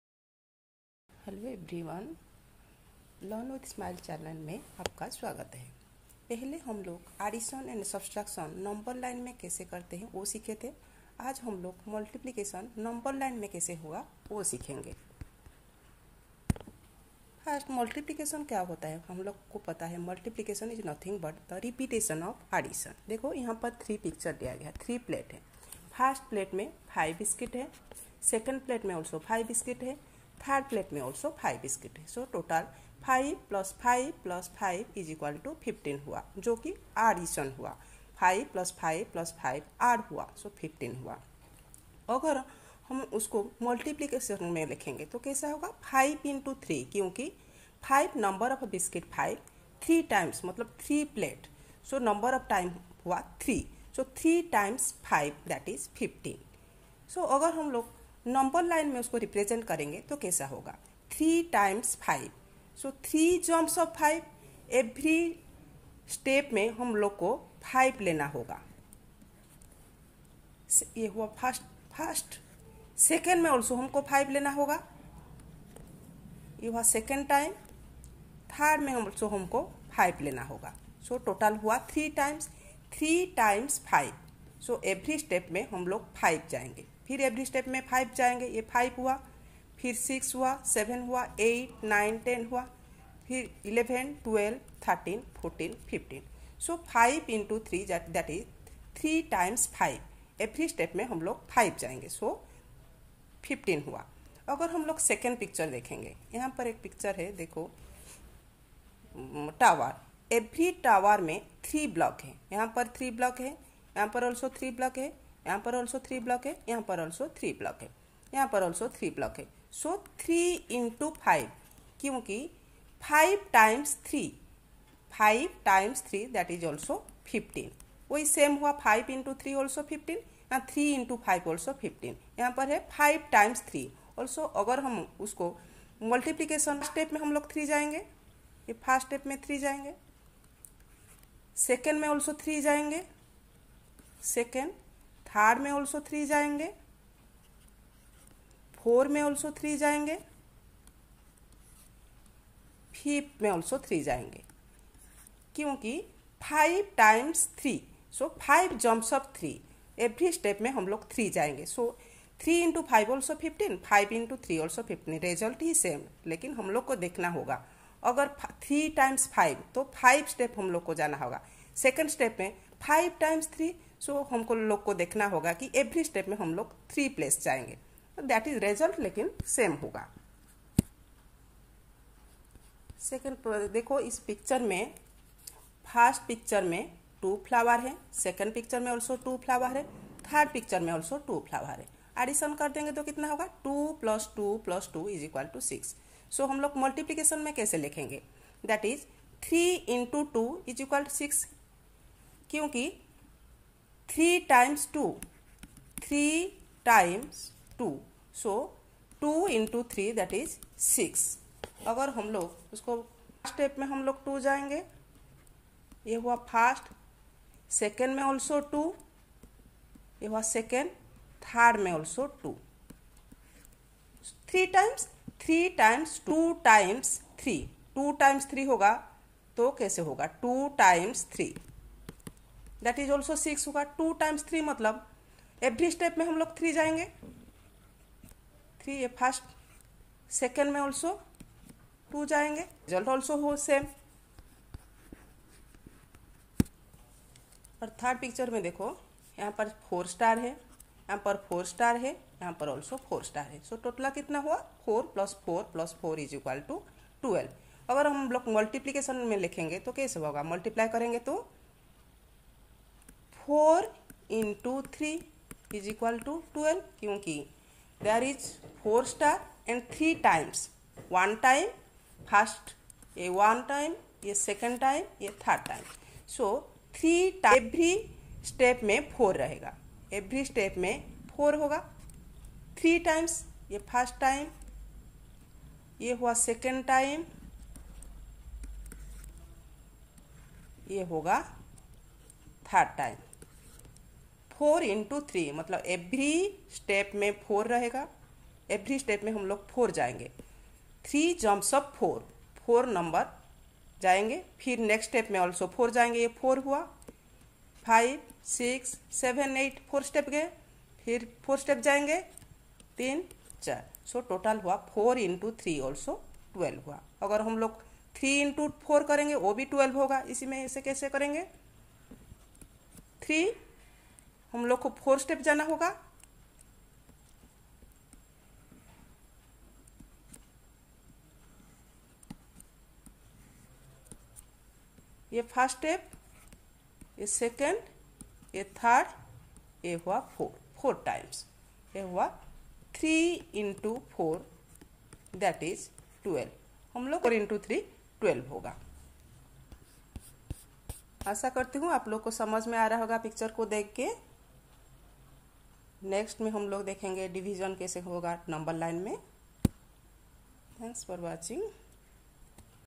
How to do multiplication on Number Line
Here, in this video, we are going to learn about multiplication on Number Line.  This video is for class primary and middle class.